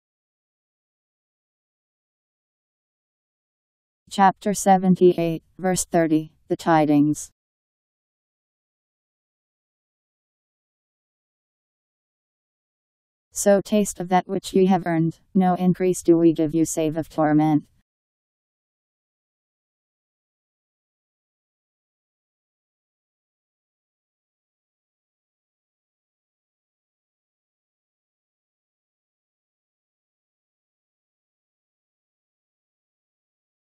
Quran The Tidings 78:30
Video shows Quran The Tidings Chapter 78 Verse 30: So taste (of that which ye have earned). No increase do We give you save of torment. Powered by MaryTTS. Holy Qur'an 78:30 Surah : Ayah